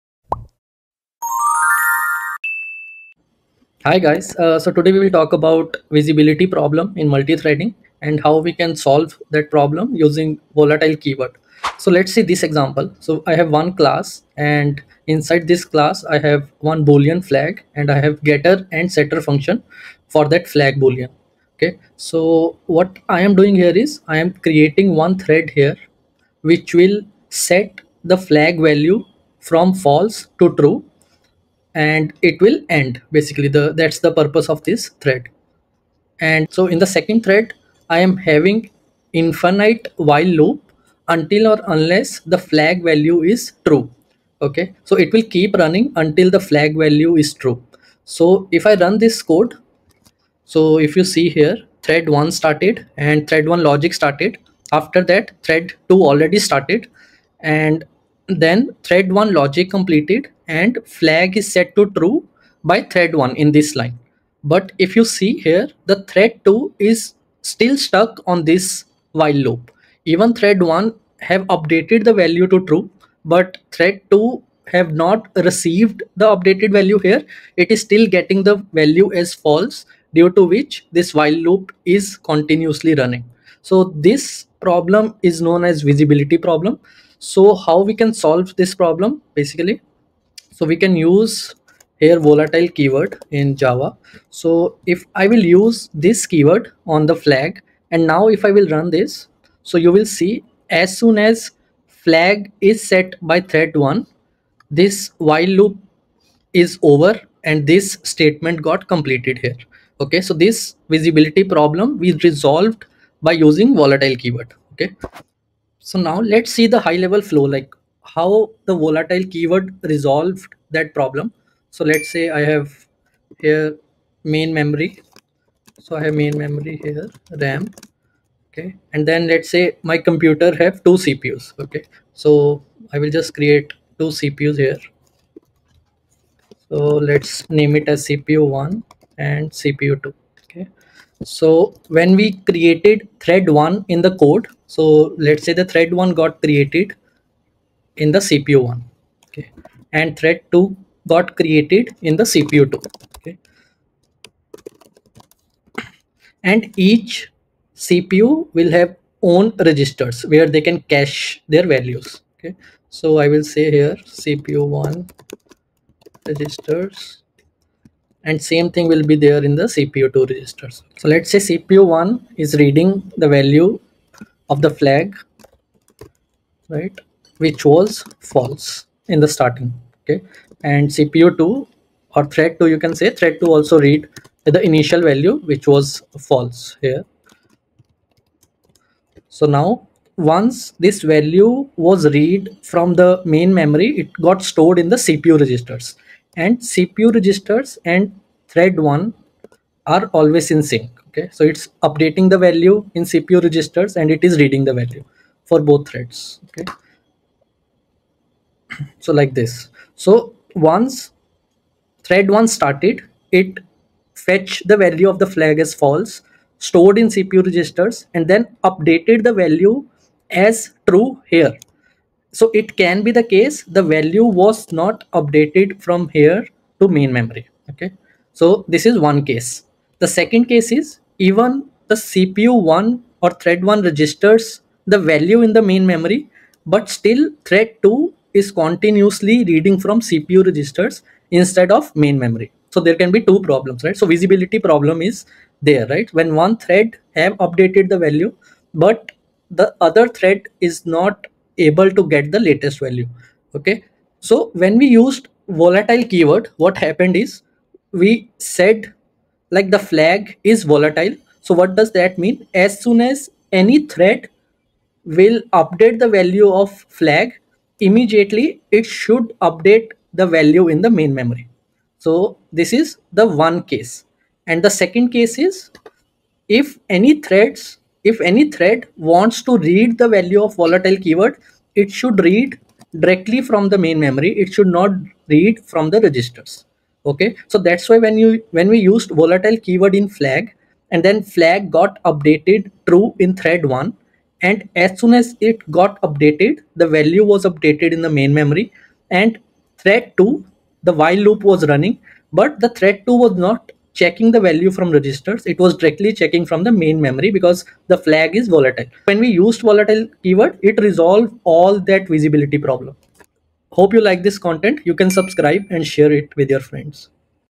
Java Multithreading: Solving Visibility Problem with Volatile Keyword
Welcome to Java Multithreading: Solving Visibility Problem with Volatile Keyword video.

In this video, dive into the fascinating world of Java multithreading and discover how to solve the visibility problem using the volatile keyword! 👩‍💻👨‍💻

What You'll Learn:

🧐 Understanding the visibility problem in multithreading.
🔍 Exploring example that highlights the need for the volatile keyword.
🛠️ Implementing the volatile keyword to ensure consistent and reliable communication between threads.
🚫 Avoiding data race conditions and achieving thread safety.

Why Watch This Video:
If you've ever encountered issues with shared variables and unpredictable behavior in your Java multithreaded programs, this video is a must-watch.

Learn how the volatile keyword acts as a powerful tool to solve visibility problems, ensuring your code behaves as expected even in a concurrent environment.